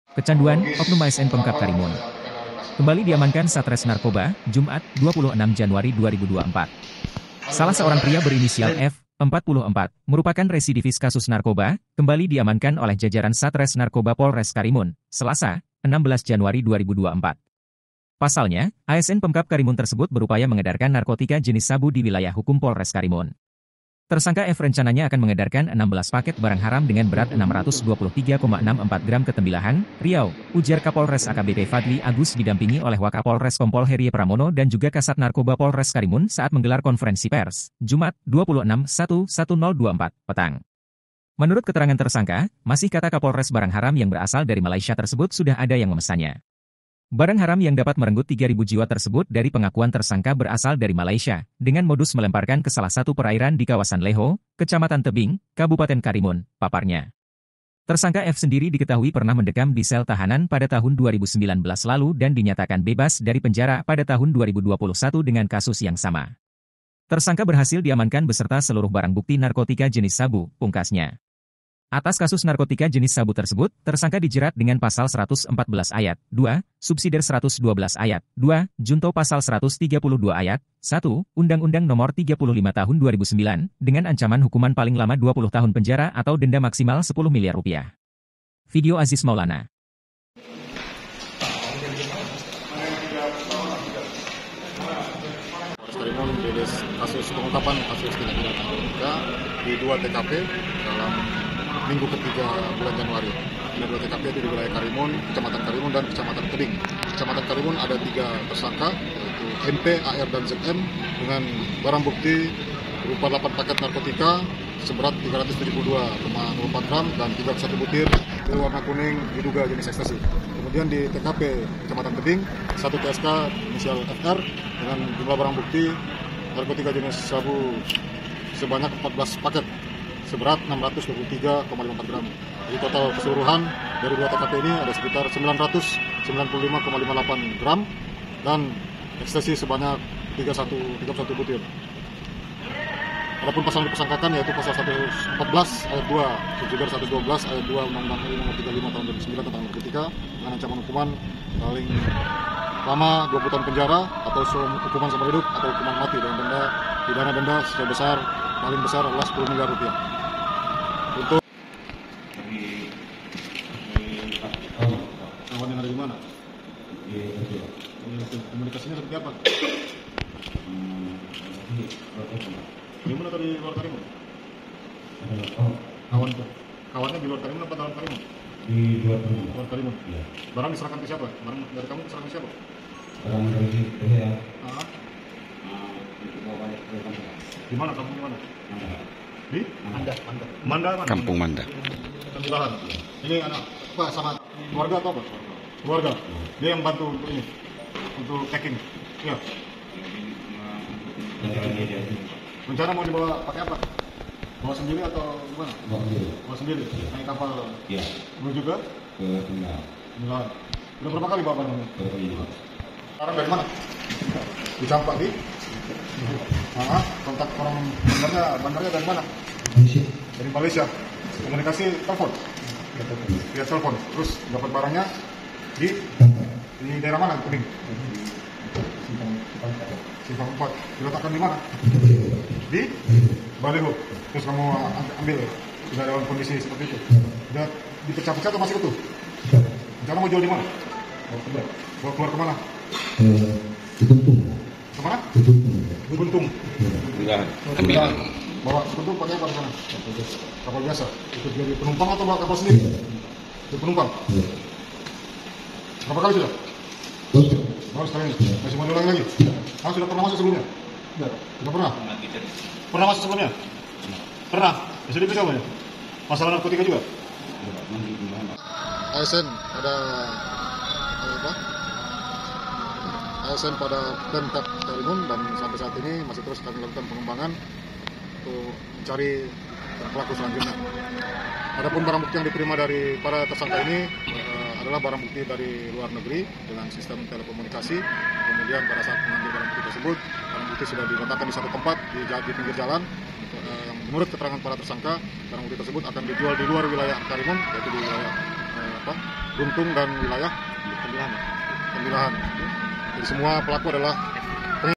Kecanduan, Oknum ASN Pemkab Karimun Kembali Diamankan Satres Narkoba Polres Karimun
wartakepritv Kecanduan, Oknum ASN Pemkab Karimun Kembali Diamankan Satres Narkoba Polres Karimun